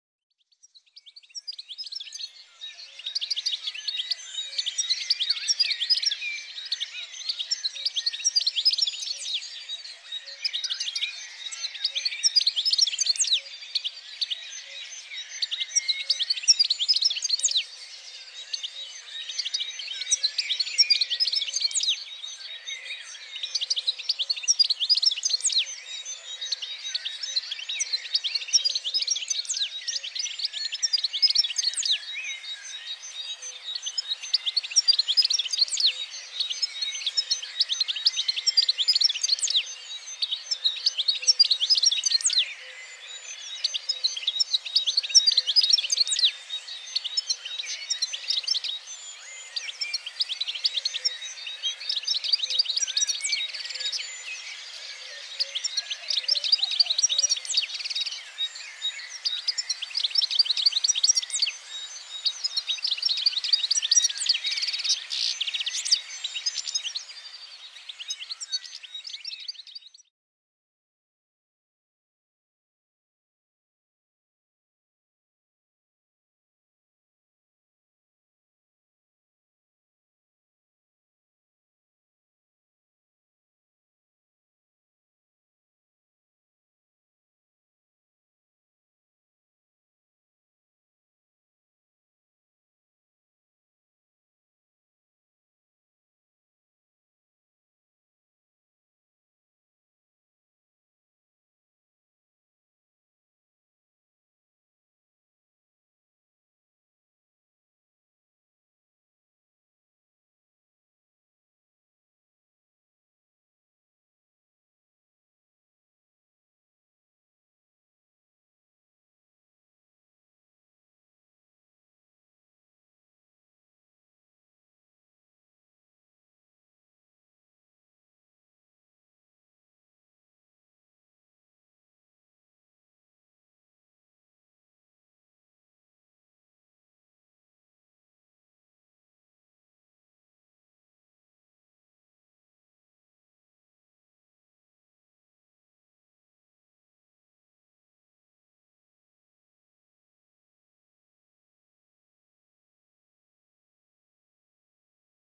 Chardonneret avril 2023
Un chardonneret sur la pelouse